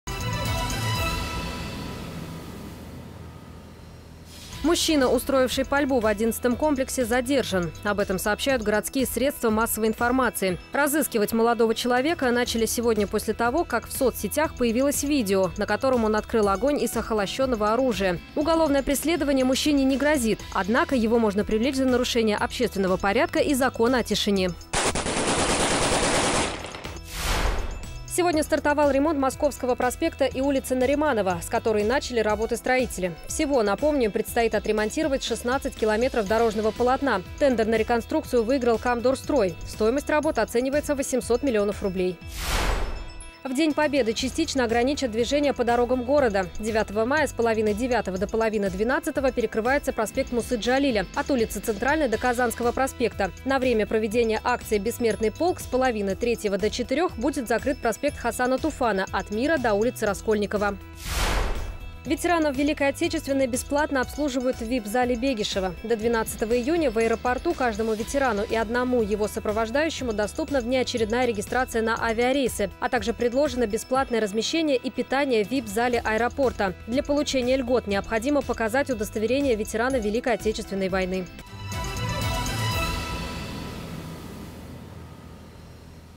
Коротко и ясно
Мужчина, устроивший пальбу в 11 комплексе, задержан. Об этом сообщают городские средства массовой информации.